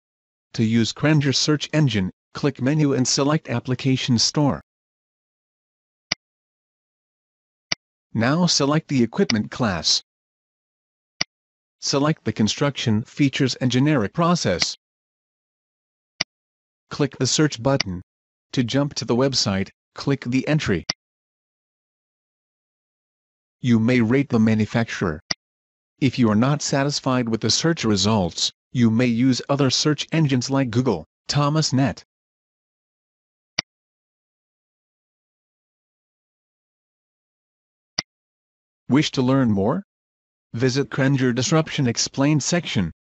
oemse
is an original equipment manufacturer (OEM) search engine. Unlike many general-purpose engines, it uses technical terminology describing the construction and the process in which the equipment searched for is used. The engine gives you a hint for most popular selections. In addition, you may score your past experience with particular manufacturer (from full satisfaction to complete miss). We plan to extend this engine to the engineering services providers